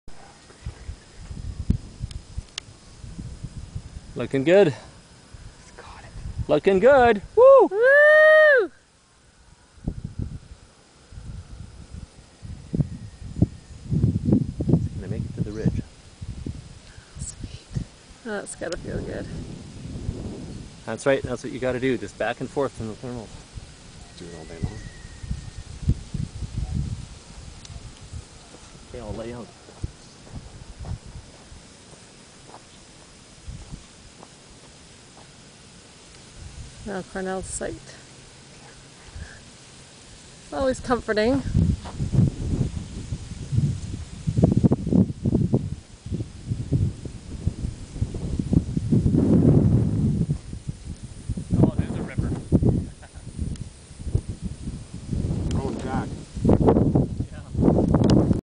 Jaco - Lee
Lee's first flight at this small, thermal site in Costa Rica